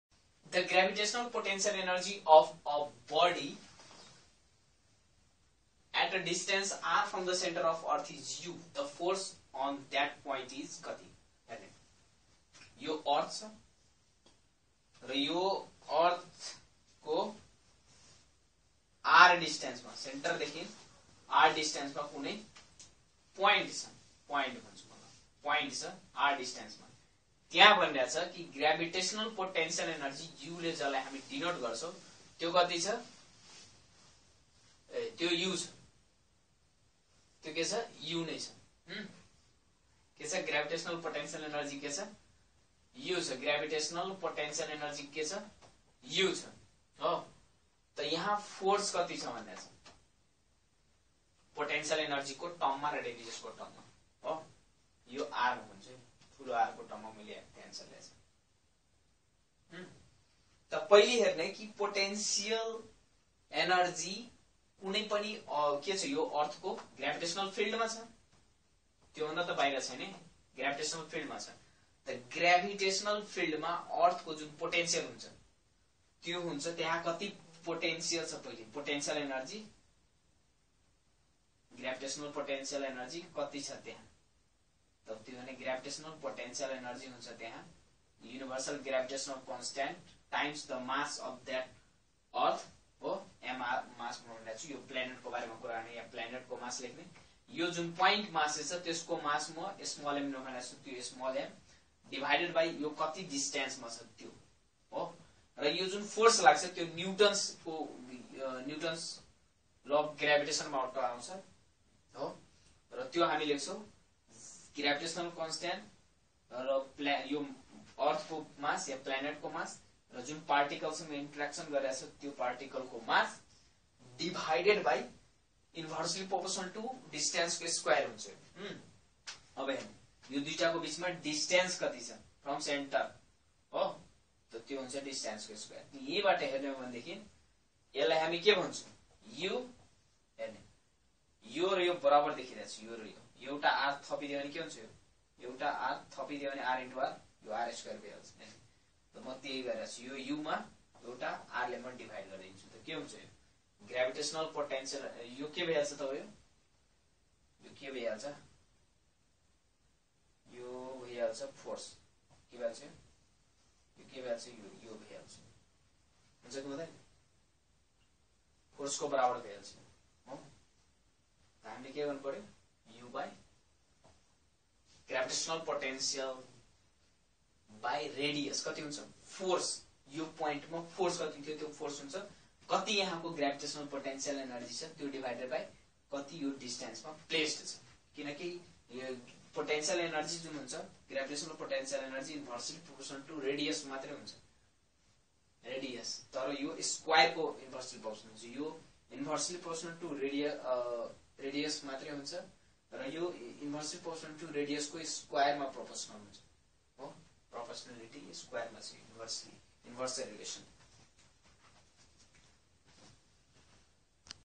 The gravitational potential energy of a body at a distance from the centre of earth is ￼. The force
The gravitational potential energy of a  body at a distance from the centre of earth is ￼. The force at that point is